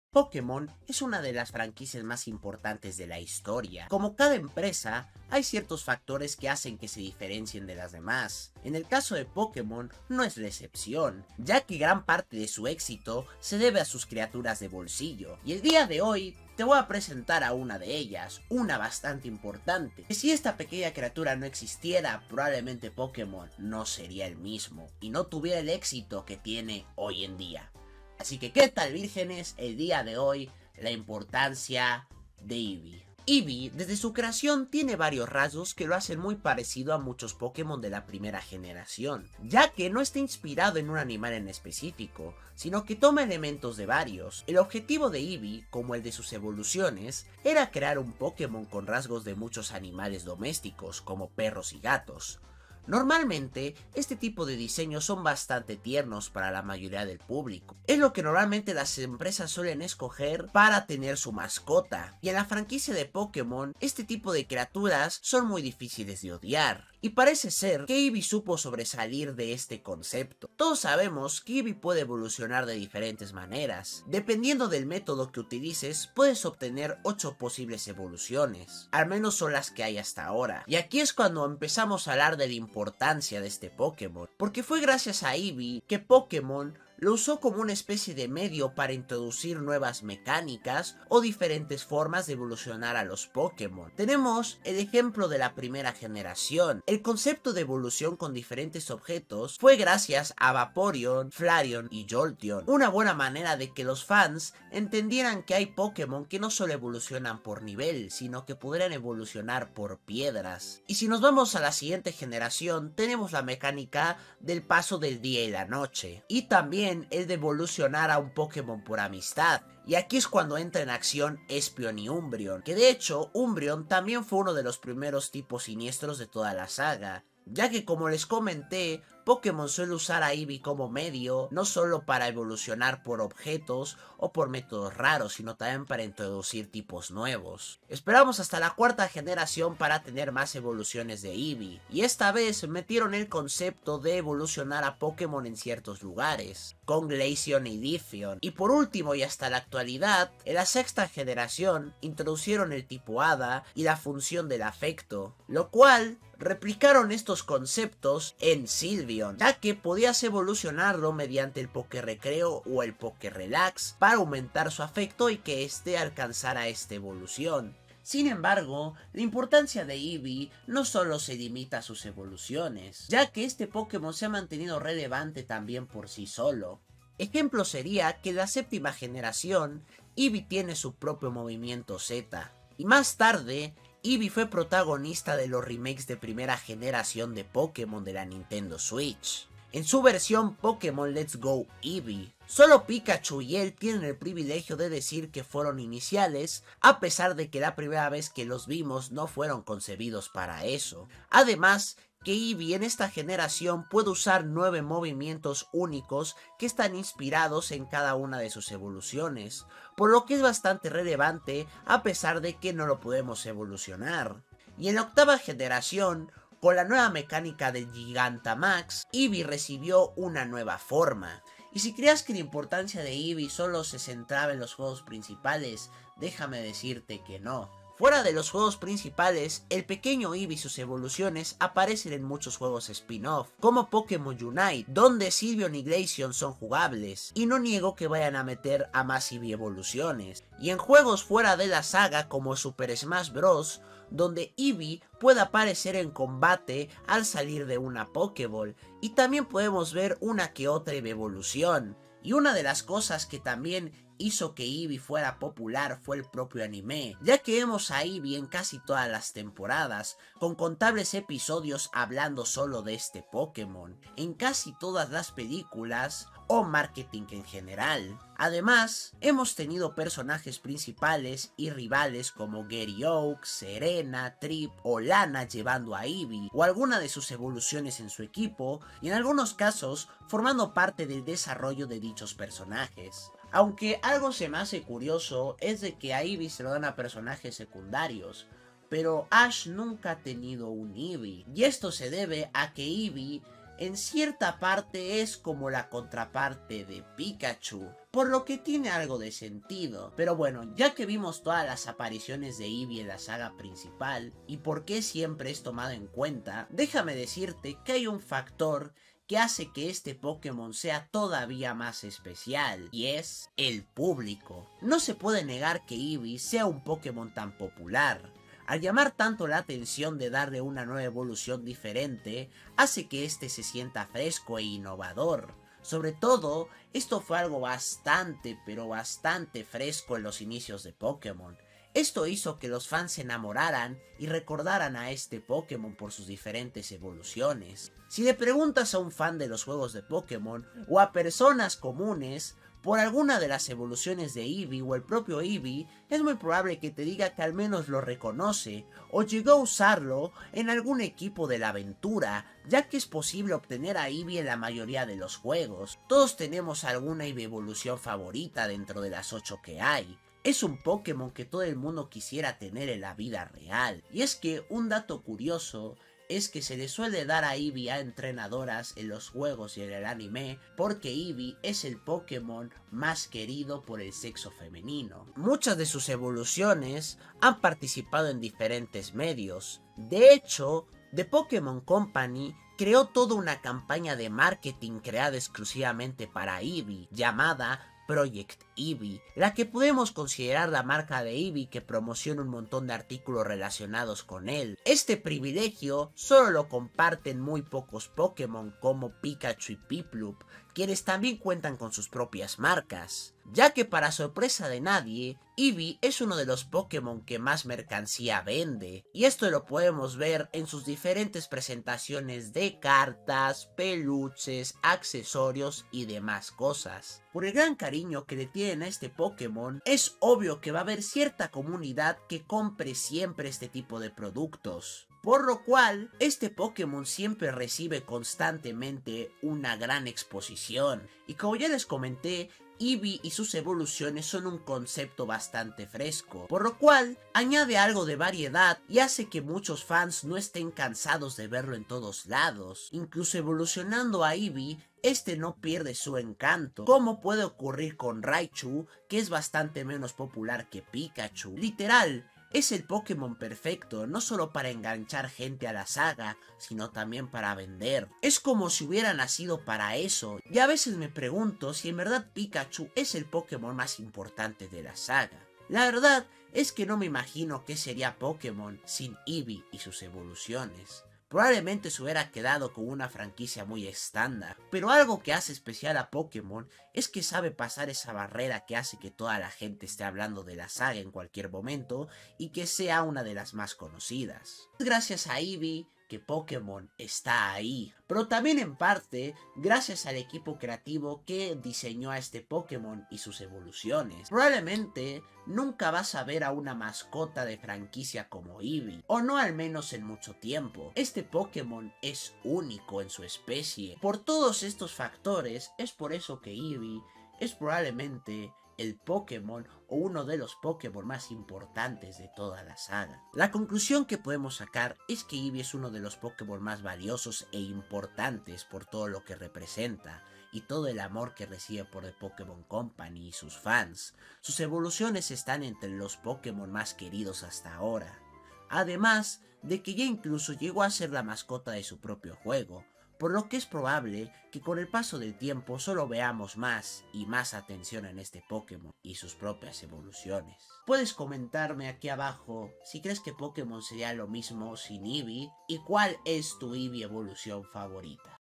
Sin EEVEE, POKEMON no seria NADA
Este video analizaremos lo importante que es Eevee para la saga de Pokemon y porque sin el esta franquicia no llegaria nada 😯😯😯